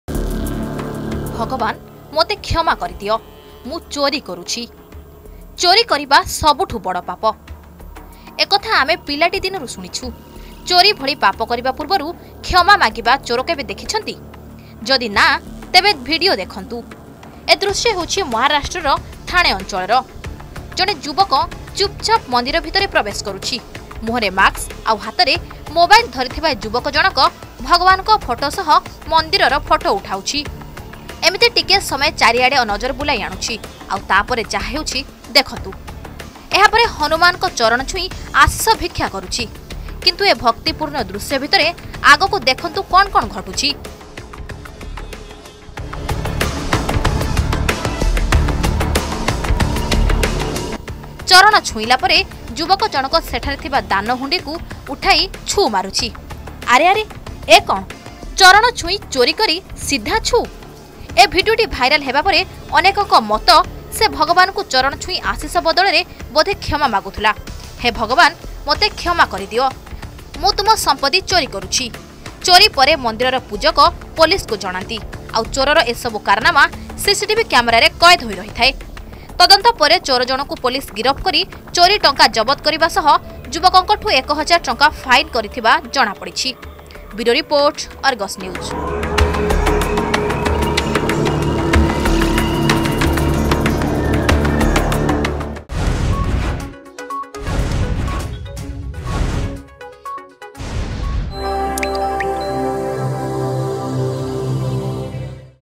ଭଗବାନ ମତେ କ୍ଷମା କରିଦିଅ, ମୁଁ ଚୋରୀ କରୁଛି
ଚୋରୀ କରିବା ସବୁଠୁ ପାପ, ଏ କଥା ଆମେ ପିଲାଟି ଦିନଠୁ ଶୁଣିଛୁ । ଚୋରୀ ଭଳି ପାପ କରିବା ର୍ପୂବରୁ କ୍ଷମା ମାଗିବା ଚୋର କେବେ ଦେଖିଛନ୍ତି, ଯଦି ନାଁ ତେବେ ଦେଖନ୍ତୁ ଏ ଭିଡିଓ ।